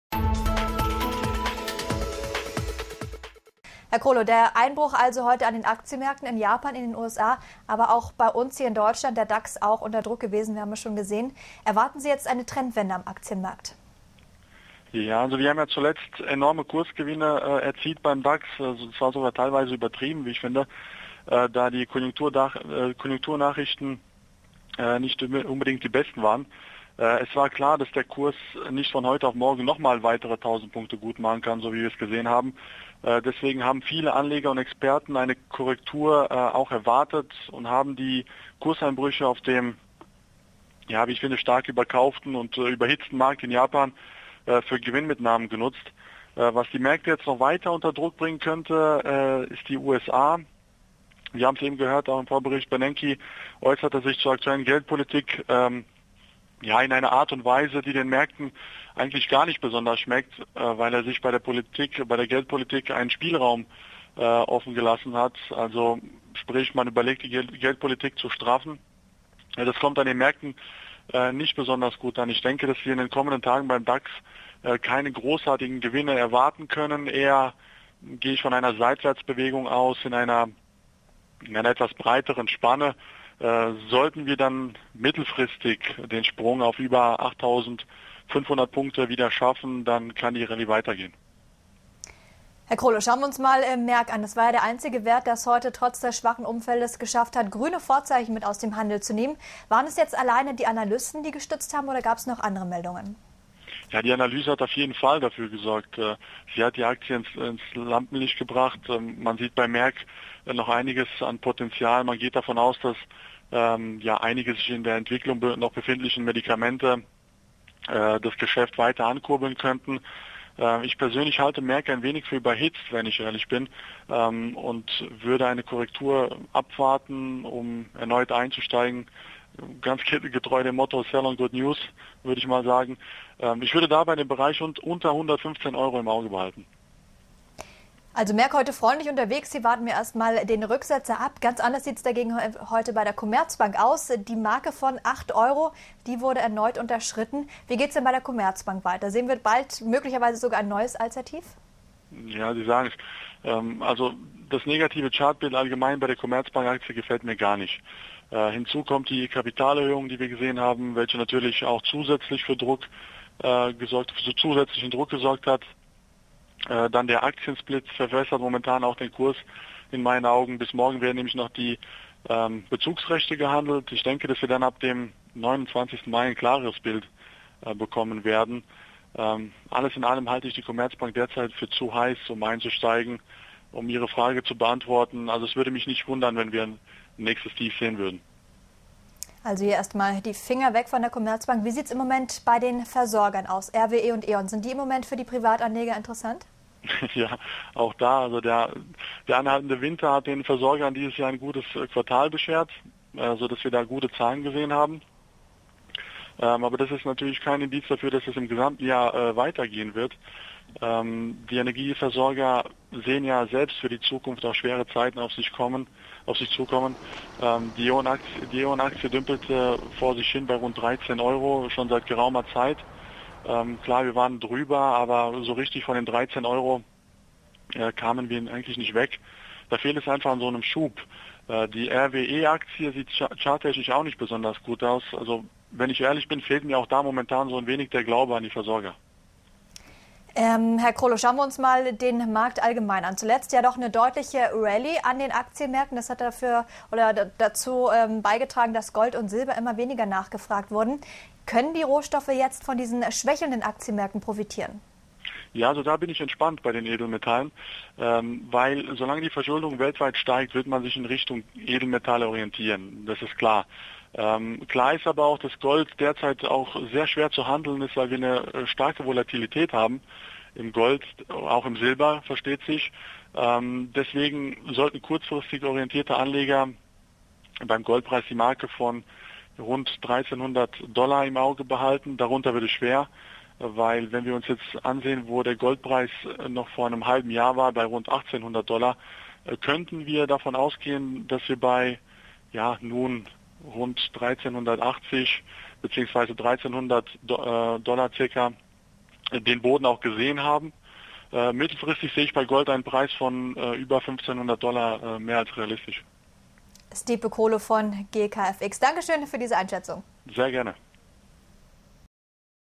Experte Krolo: USA könnten Märkte weiter unter Druck
Im Interview blickt DAF-Moderatorin Karolin Oltersdorf zusammen mit Stipe Krolo vom Brokerhaus GKFX auf den heutigen Handelstag zurück. Dabei wird nicht nur über die heftigen Kursverluste an den internationalen Börsen diskutiert, sondern auch über die Aktien von Merck und der Commerzbank.